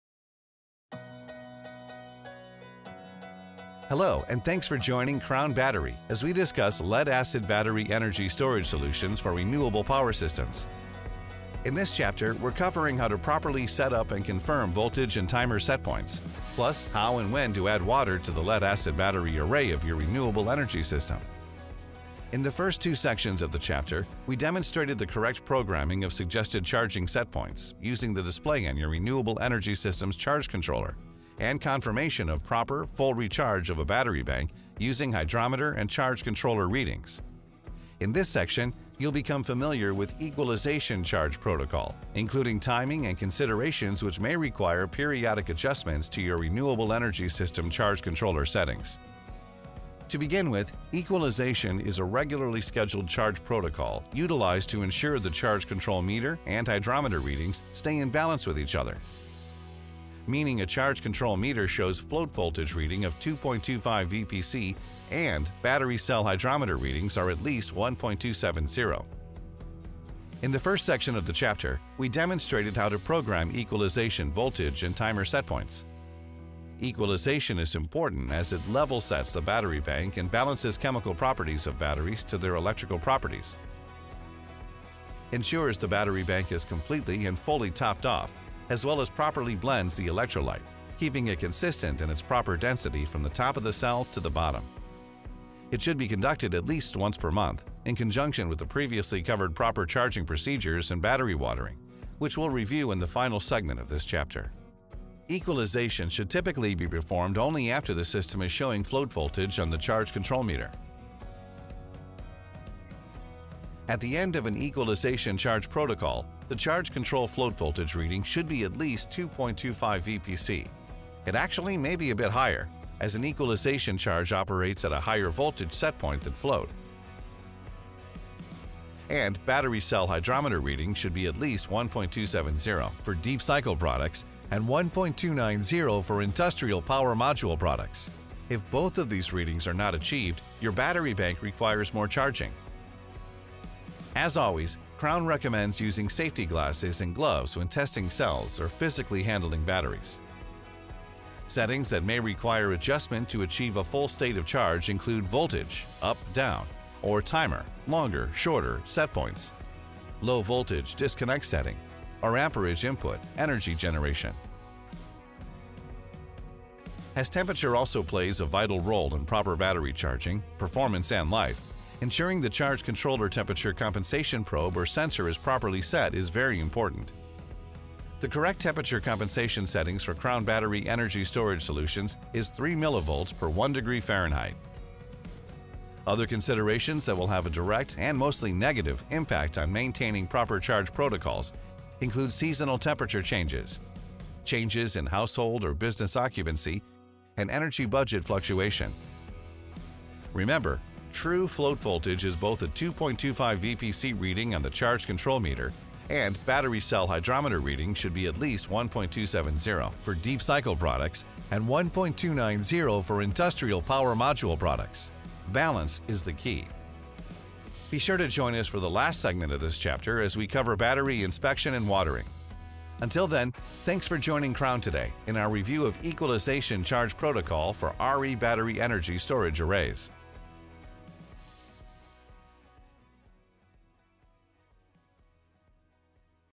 Equalizing Charge Protocol
In this video, you’ll become familiar with equalization charge protocol, including timing and considerations which may require periodic adjustments to your renewable energy system charge controller settings.